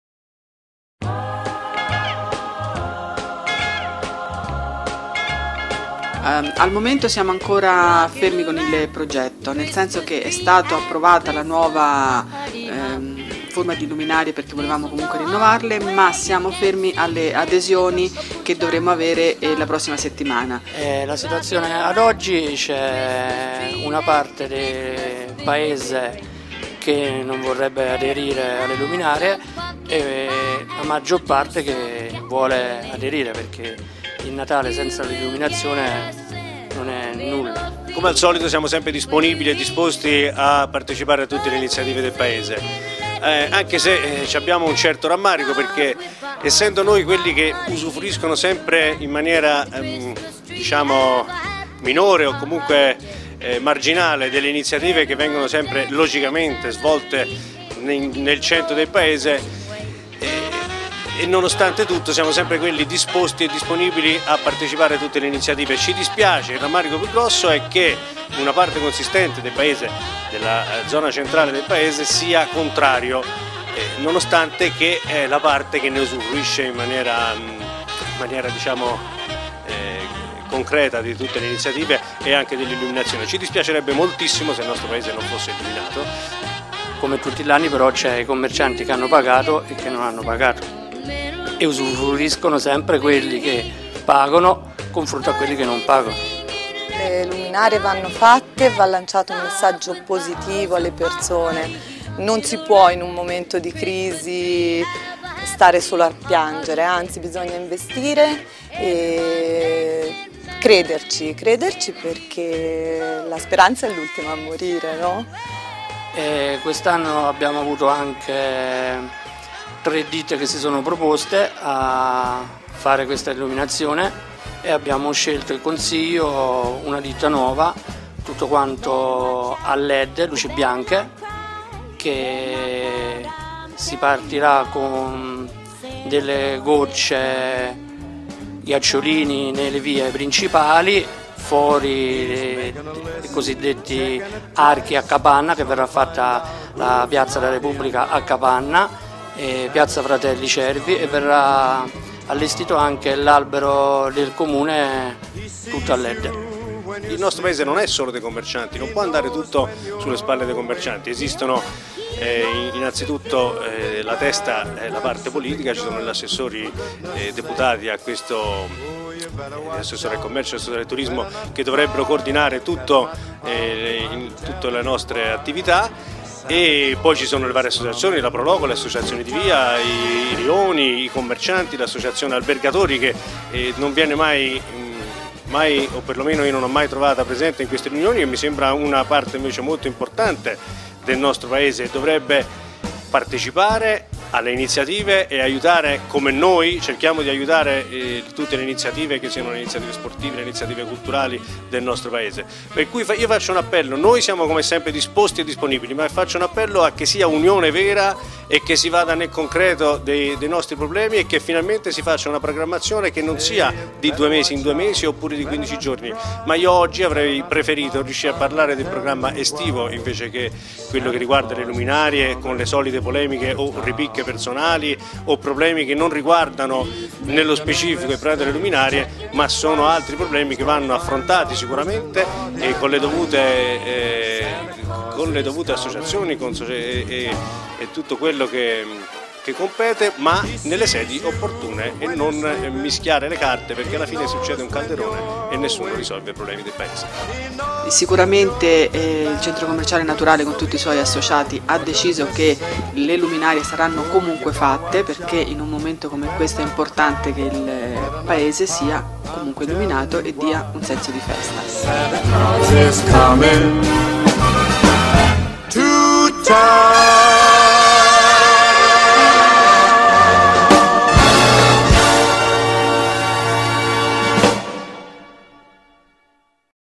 Abbadia s.s. luminarie Natale 2011. Interviste ai commercianti
Interviste ai commercianti di Abbadia San Salvatore sulle luminarie di Natale 2011.